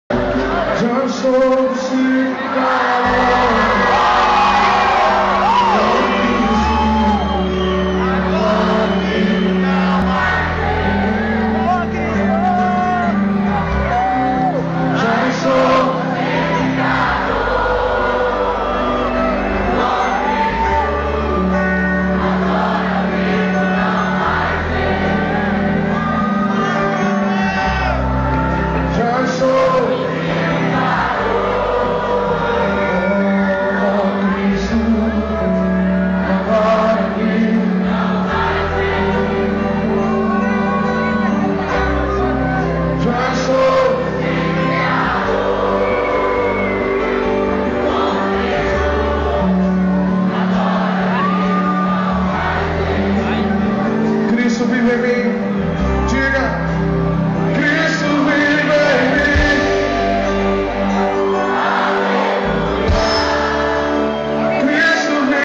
Show do Fernandinho - DDD 25/10/2008
Show do Fernandinho em Brasília - Dia de decidir destinos - com Luan, Priscila, Walisson, Dheysson e Diego.